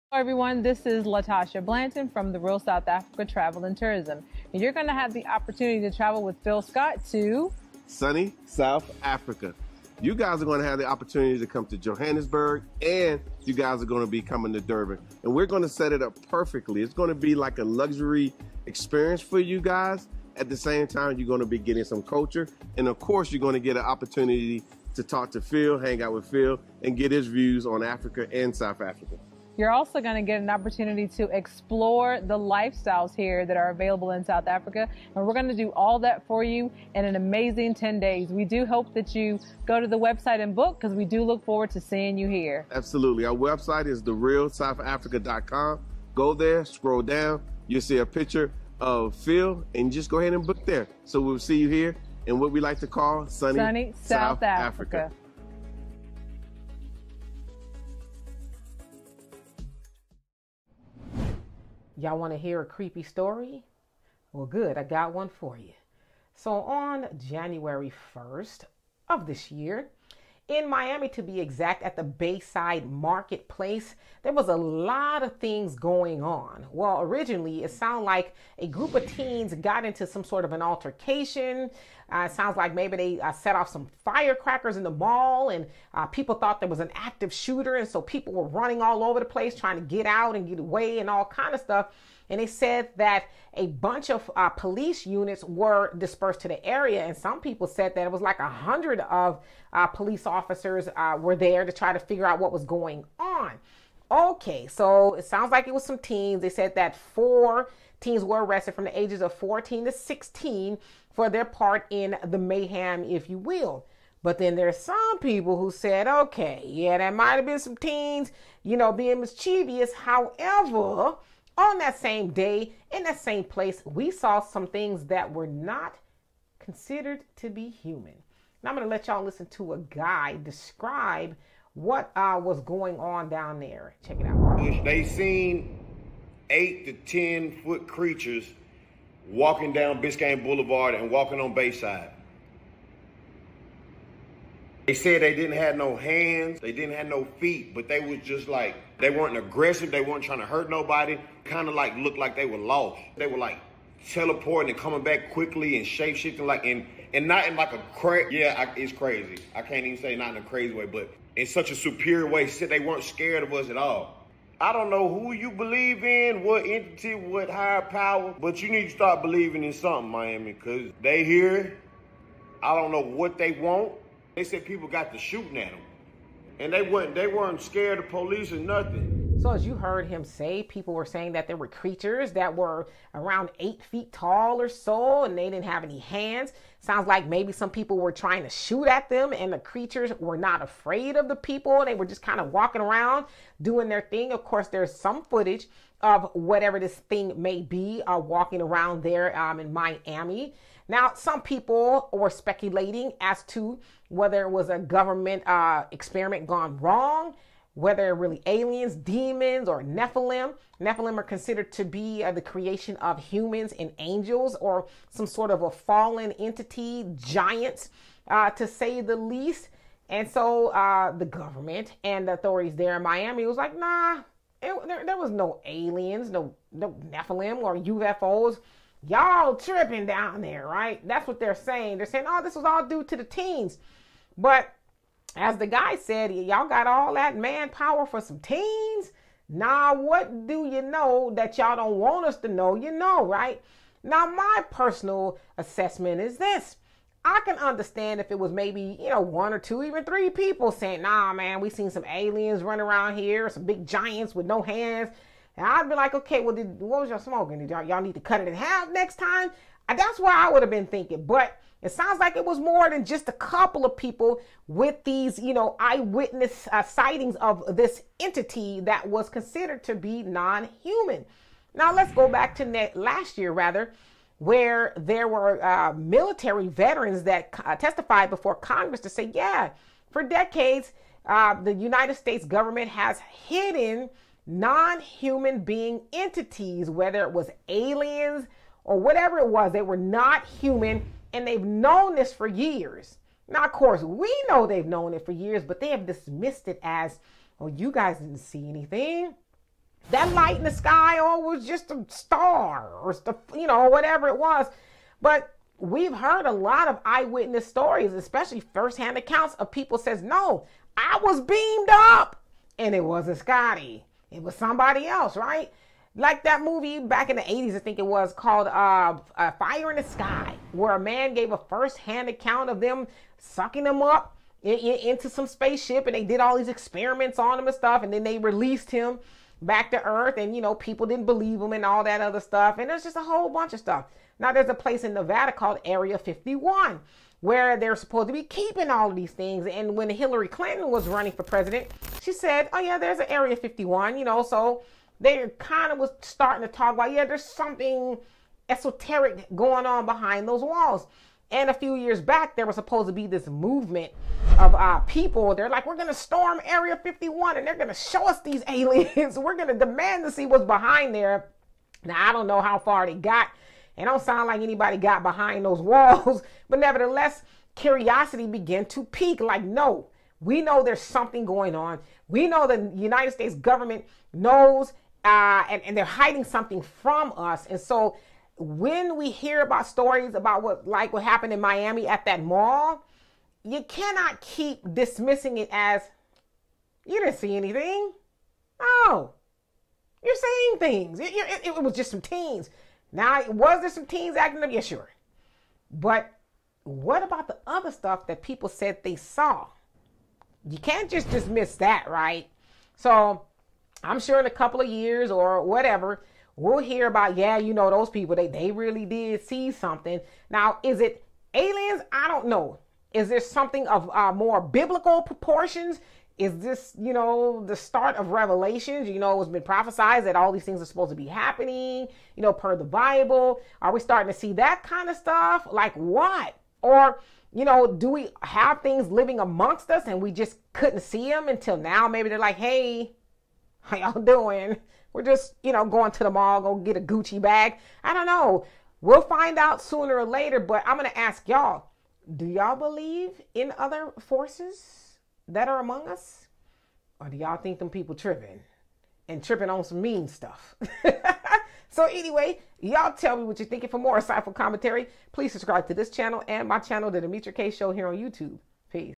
Sightings Of Giant Creatures In Miami Terrify Mall-Goers, But Authorities Deny The Occurrence
Demetra Kaye reports on sightings of giant creatures in Miami terrifying mall-goers, but authorities deny such occurrence.